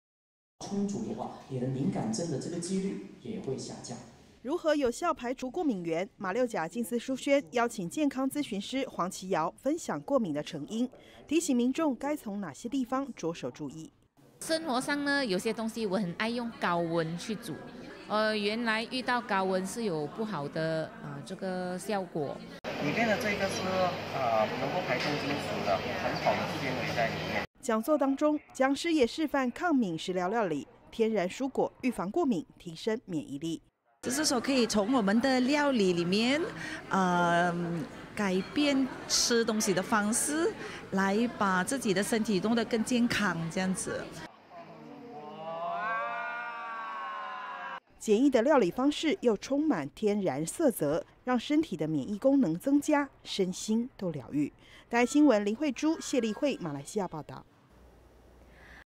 適當烹調.吃對蔬果 抗過敏隨手就能做
空氣汙染加上外食習慣，有過敏體質的人口就相對增加。慈濟馬六甲靜思書軒，舉辦健康講座，邀請健康諮詢師，分享過敏的成因和抗敏食物的調理。讓民眾對困惱已久的過敏體質，尋求到一帖良方。

健康諮詢師 黃其銚：「你消化充足的話 你的敏感症，(發作)的機率也會下降。」

如何有效排除過敏原，馬六甲靜思書軒，邀請健康諮詢師黃其銚，分享過敏的成因，提醒民眾該從哪些地方著手注意。

民眾 王靖風：「生活上有些東西，我很愛用高溫去煮，原來遇到高溫 是有不好的效果。」

講座當中，講師也示範抗敏食療料理，天然蔬果預防過敏，提升免疫力。

民眾 符慧敏：「就是可以從我們的料理裡面，改變吃東西的方式，來把自己的身體弄得更健康。」

簡易的料理方式，又充滿天然色澤，讓身體的免疫功能增加，身心都療癒。

馬來西亞報導：林惠珠 謝麗惠